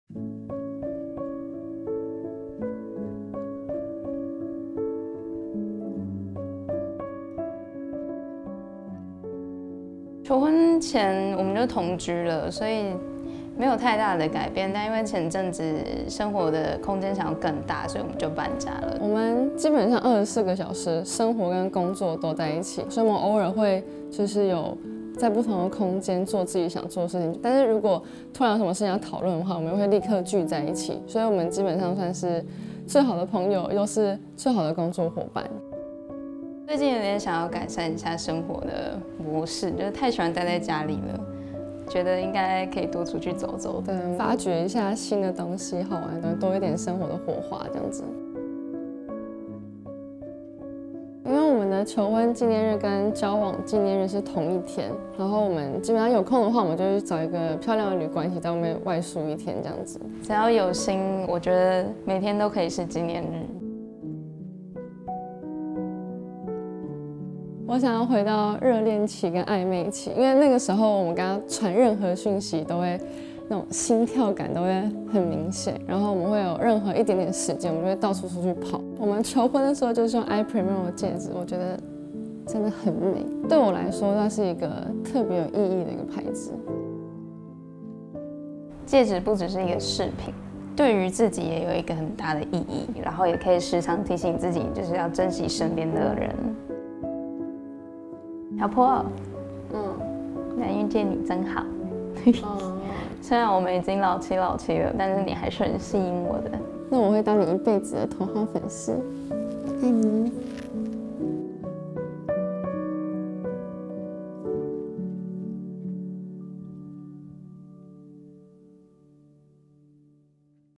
結婚周年交換I-PRIMO紀念戒：每一天都是值得紀念的珍貴時刻｜卡卡&貝拉｜ELLE Taiwan
卡卡和貝拉，一位原本從事服裝設計及模特兒產業，一位是直播主，兩個漂亮的女孩，在交友平台認識。在同婚兩周年之際，卡卡與貝拉也戴上I-PRIMO的周年紀念戒，她們很感謝自己生在台灣，是亞洲第一個通過同婚的國家，對她們而言，戒指不只是一個飾品，而有著很大的意義，可以時常提醒自己，要珍惜身邊的人。